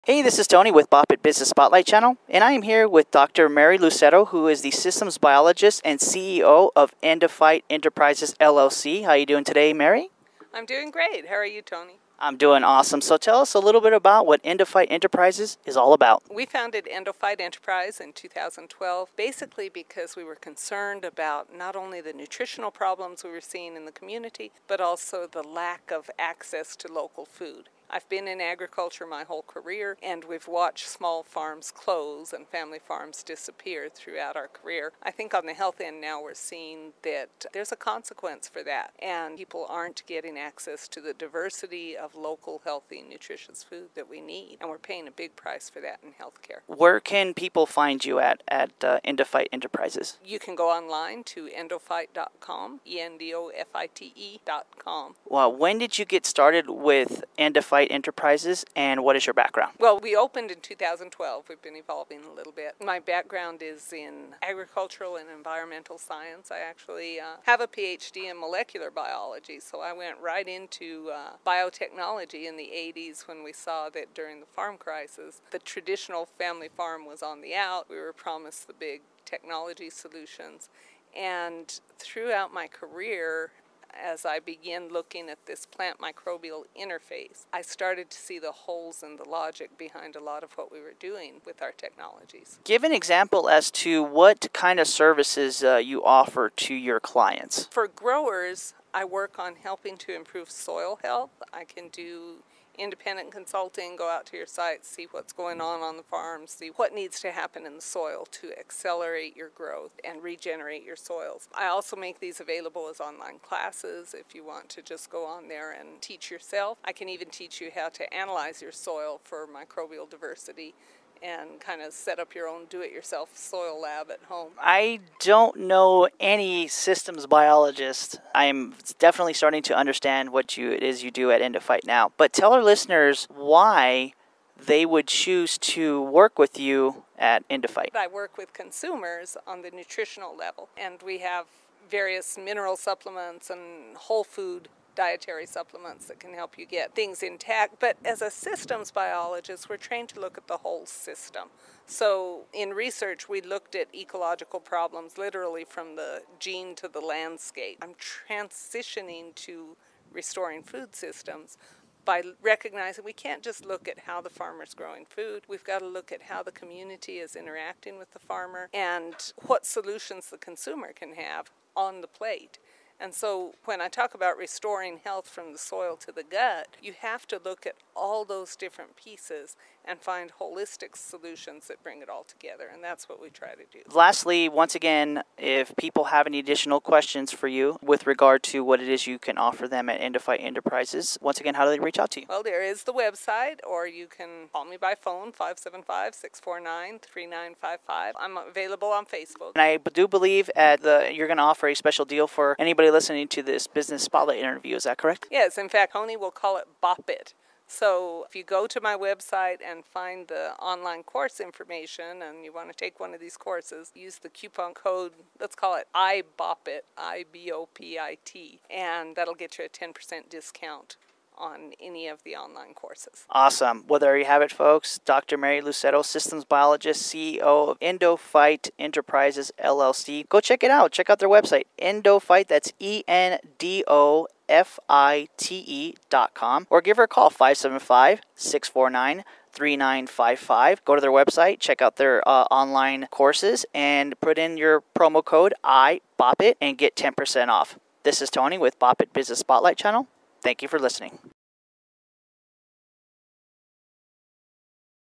End-O-Fite Enterprises LLC
This business spotlight features Dr. Mary Lucero who is the Systems Biologist and CEO of End-O-Fite Enterprises LLC. Mary discusses her career while working for the U.S. Department of Agriculture. Mary explains the issues that small farms are facing and what she is doing to help restore microbiomes in the crops that are grown. These microbiomes benefits the food we eat everyday that contributes to our overall well being. Mary also explains the various services she offers to both farmers and consumers such as consulting and educational courses.&nbsp;